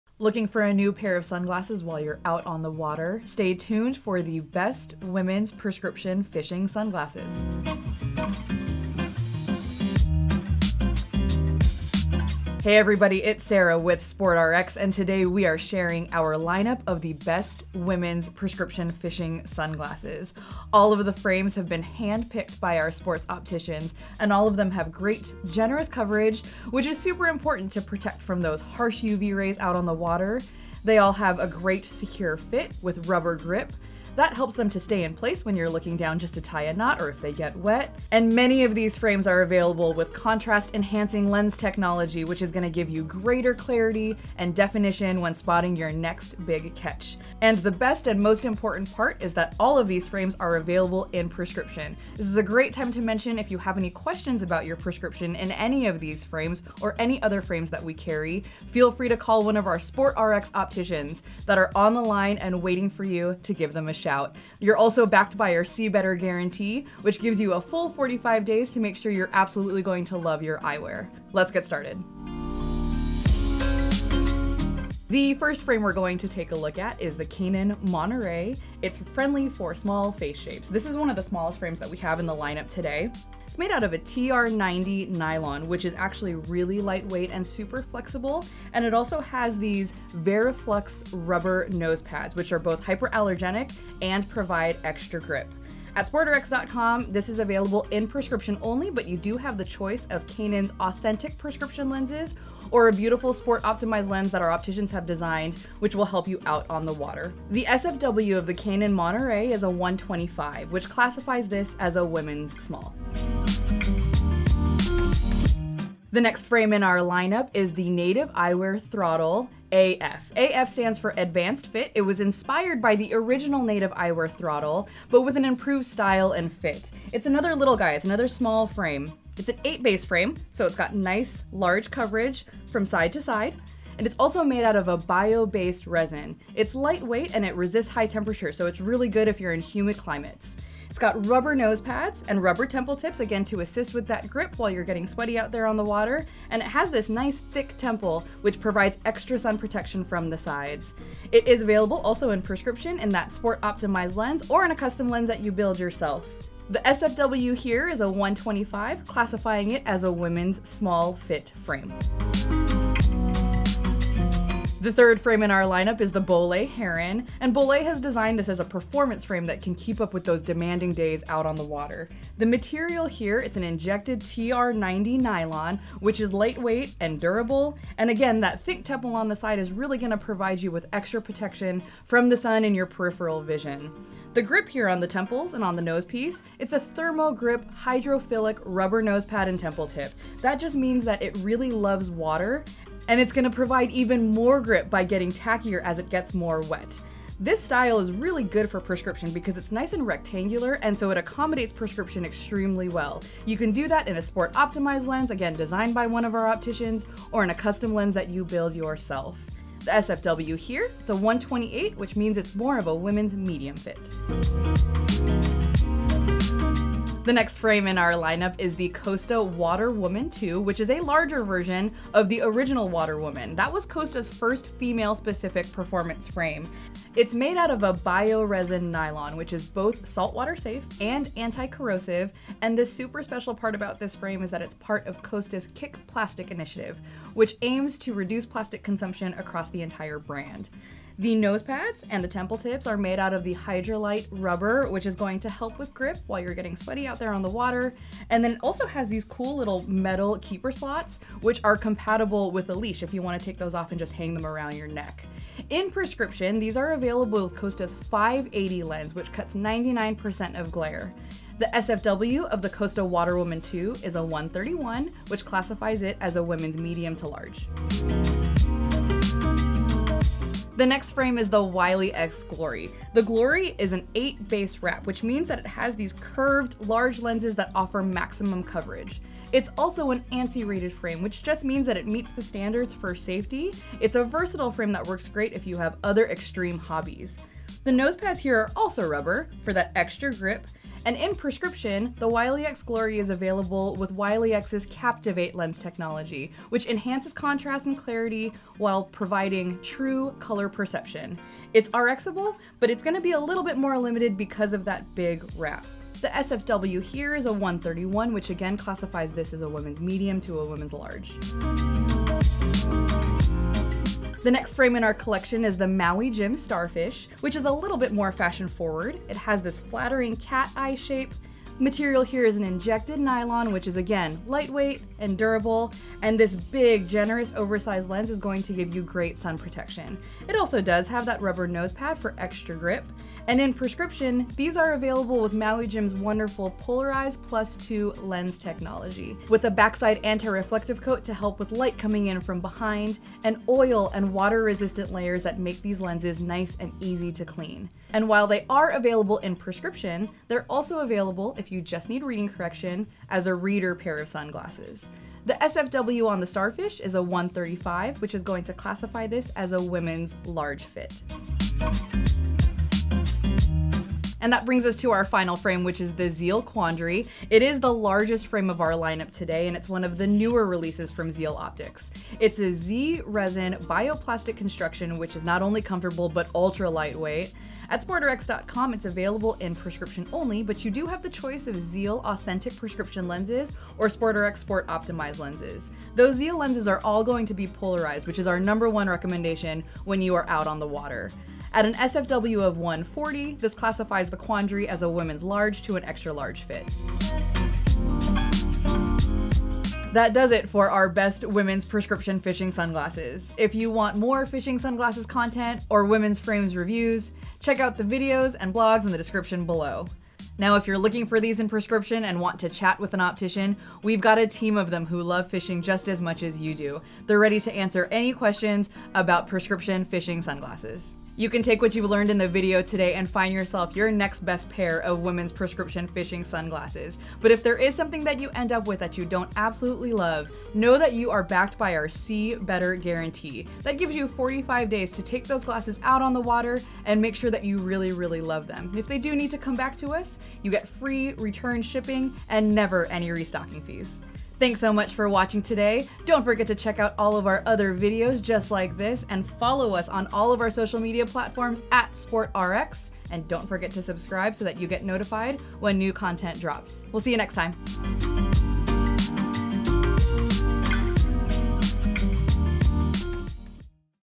Top 7 Best Prescription Fishing Sunglasses for Women of 2021 | SportRx
Tired of carrying two frames out on the boat when you go fishing? Say goodbye to days of forgetting your eyeglasses on land with the convenience of prescription fishing sunglasses! Join SportRx for our lineup of the Best Women’s Prescription Fishing Sunglasses!

0:13 Intro & Common Features
1:12 Kaenon Monterey
1:54 Native Throttle AF
2:42 Bolle Heron
3:34 Costa Waterwoman II
4:28 Wiley x Glory
5:12 Maui Jim Starfish
6:02 Zeal Quandary
6:38 Conclusion

Worried about shopping online for prescription glasses?
Don’t know your size? Measure with the SportRx SFW
Questions about your prescription?